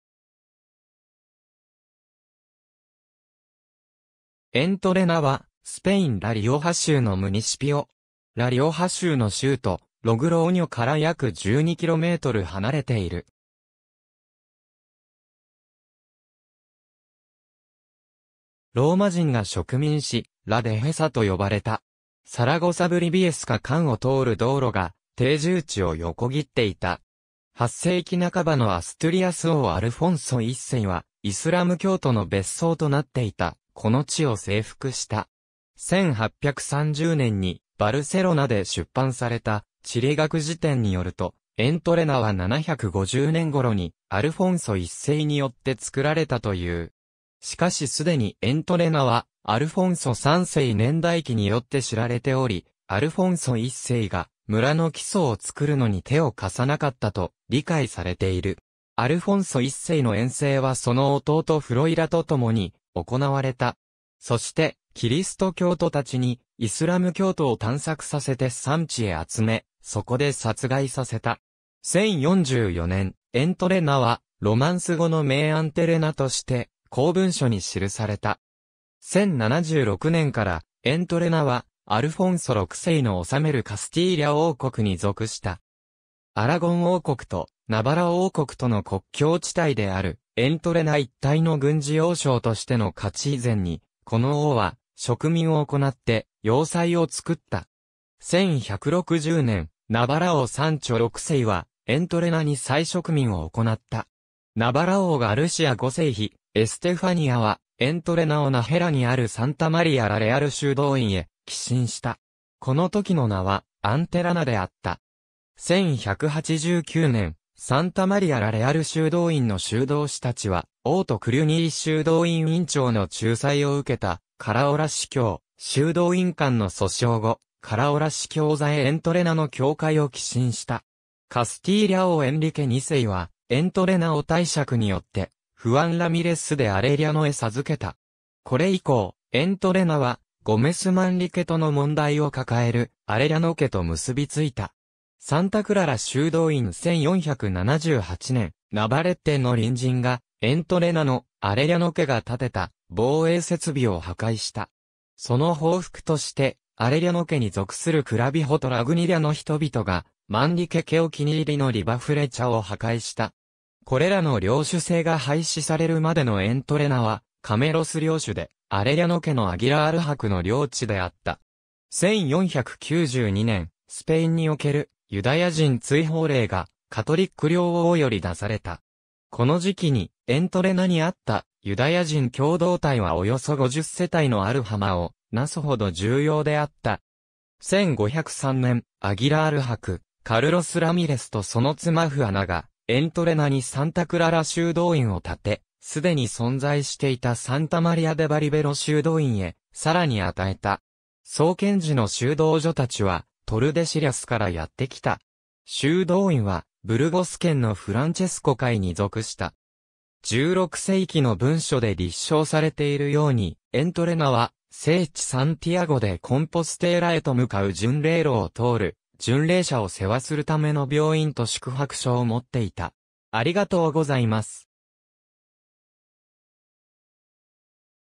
エントレナ
ラ・リオハ州の自治体
エントレナ（スペイン語: Entrena）は、スペイン・ラ・リオハ州のムニシピオ（基礎自治体）。
ラ・リオハ州の州都ログローニョから約12km離れている。
ローマ人が植民し、ラ・デヘサ(La Dehesa)と呼ばれた。
サラゴサ＝ブリビエスカ間を通る道路が定住地を横切っていた。
8世紀半ばのアストゥリアス王アルフォンソ1世は、イスラム教徒の別荘となっていたこの地を征服した。
1830年にバルセロナで出版された地理学辞典によると、エントレナは750年頃にアルフォンソ1世によってつくられたという。
しかし既にエントレナは「アルフォンソ3世年代記」によって知られており、アルフォンソ1世が村の基礎を創るのに手を貸さなかったと、理解されている。
アルフォンソ1世の遠征はその弟フロイラとともに行われた。
そしてキリスト教徒たちにイスラム教徒を探索させて山地へ集め、そこで殺害させた。
1044年、エントレナはロマンス語の名アンテレナ(Antelena)として公文書に記された。
1076年からエントレナはアルフォンソ6世の治めるカスティーリャ王国に属した。
アラゴン王国とナバラ王国との国境地帯であるエントレナ一帯の軍事要衝としての価値以前に、この王は植民を行って要塞をつくった。
1160年、ナバラ王サンチョ6世は、エントレナに再植民を行った。
ナバラ王ガルシア5世妃エステファニアは、エントレナをナヘラにあるサンタ・マリア・ラ・レアル修道院へ寄進した。
この時の名はアンテラナ(Antelana)であった。
1189年、サンタ・マリア・ラ・レアル修道院の修道士たちは、王とクリュニー修道院院長の仲裁を受けたカラオラ司教・修道院間の訴訟後、カラオラ司教座へエントレナの教会を寄進した。
カスティーリャ王エンリケ2世はエントレナを貸借によってフアン・ラミレス・デ・アレリャノへ授けた。
これ以降エントレナは、ゴメス・マンリケ（近郊の自治体ナバレッテ領主）との問題を抱えるアレリャノ家と結びついた。
サンタ・クララ修道院 1478年、ナバレッテの隣人が、エントレナのアレリャノ家が建てた防衛設備を破壊した。
その報復として、アレリャノ家に属するクラビホとラグニリャの人々が、マンリケ家お気に入りのリバフレチャを破壊した。
これらの領主制が廃止されるまでのエントレナは、カメロス領主でアレリャノ家のアギラール伯の領地であった。
1492年、スペインにおけるユダヤ人追放令がカトリック両王より出された。
この時期にエントレナにあったユダヤ人共同体はおよそ50世帯のアルハマ（ユダヤ人共同体）をなすほど重要であった。
1503年、アギラール伯カルロス・ラミレスとその妻フアナが、エントレナにサンタ・クララ修道院を建て、既に存在していたサンタ・マリア・デ・バリベロ修道院へさらに与えた。
創建時の修道女たちはトルデシリャスからやってきた。
修道院はブルゴス県のフランチェスコ会に属した。
16世紀の文書で立証されているように、エントレナは、聖地サンティアゴ・デ・コンポステーラへと向かう巡礼路を通る巡礼者を世話するための病院と宿泊所を持っていた。